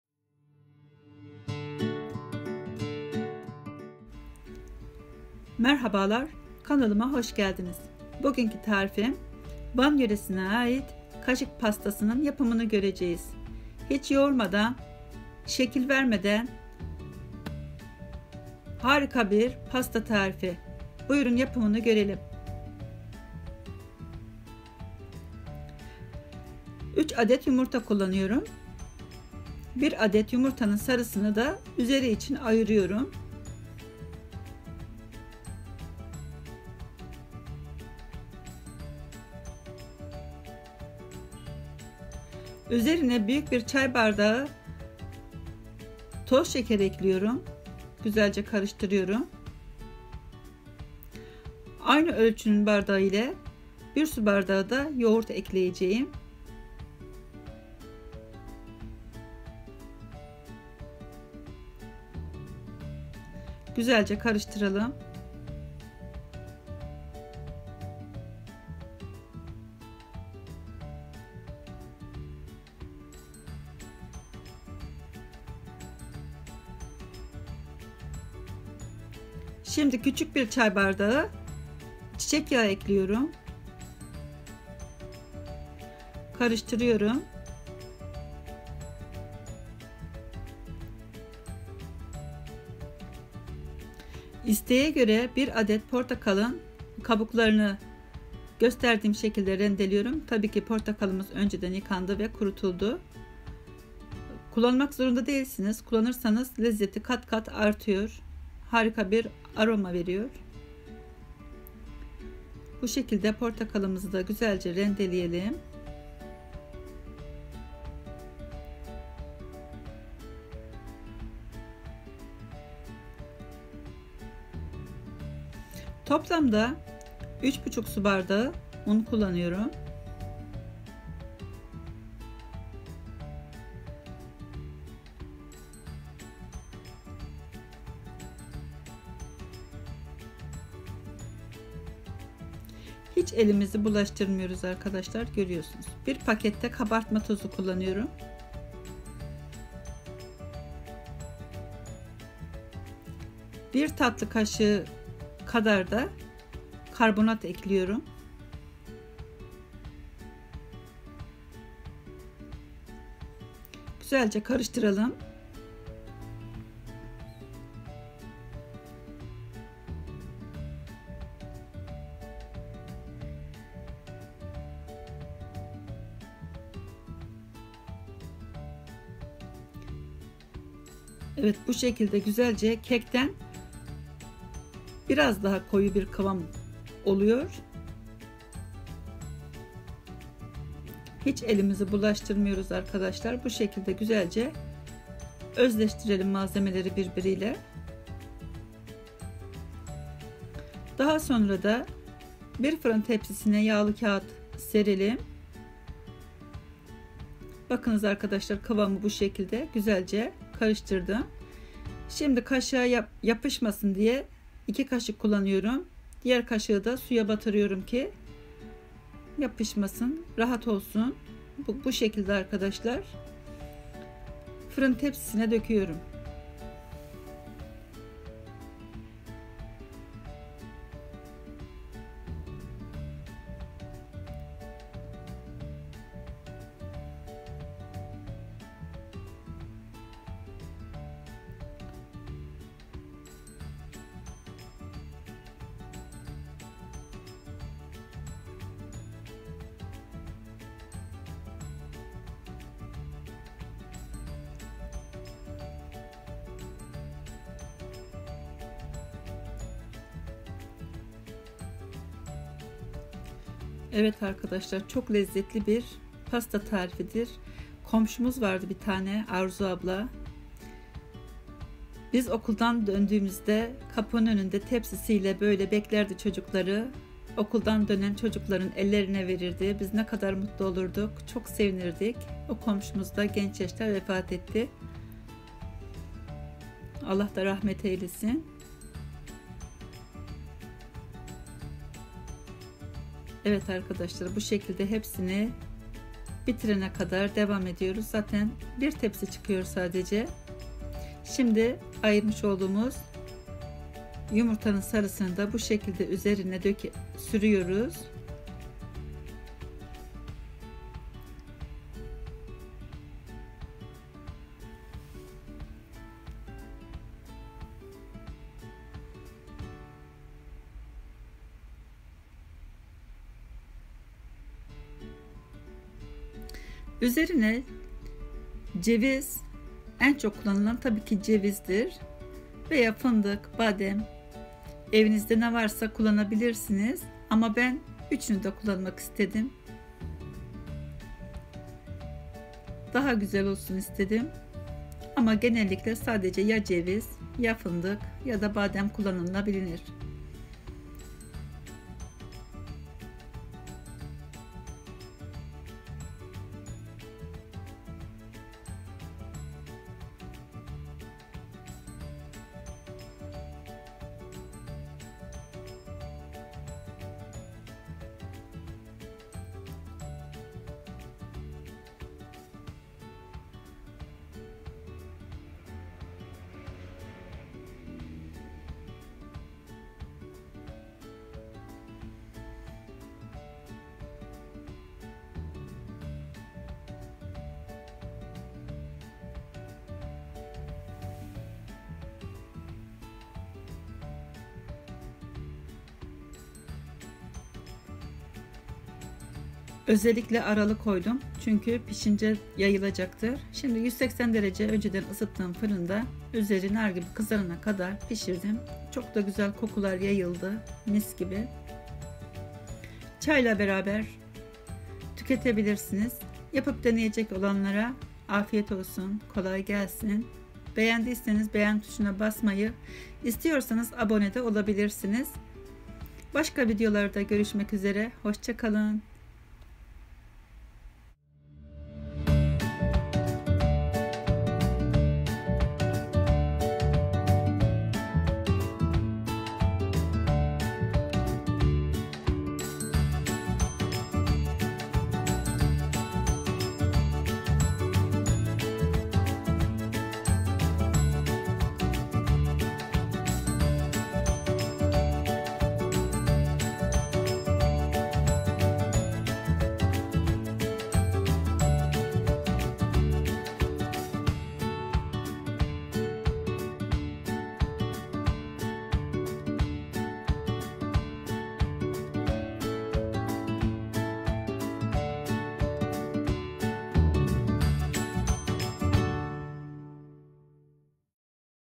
YOĞURMAK YOK ELİNİZİ SÜRMEDEN PASTA/VAN KAŞIK PASTASI TARİFİ/VAN KURABİYESİ/yemek adası
beni instagram hesabımdan takip edebilirsiniz

malzemeler
üç adet yumurta
bir çay fincanı şeker
bir çay fincanı yoğurt
bir çay bardağı sıvı yağ
bir adet portakal rendesi
üç buçuk su bardağı un
bir paket kabartma tozu
bir tatlı kaşığı karbonat
detayları videoda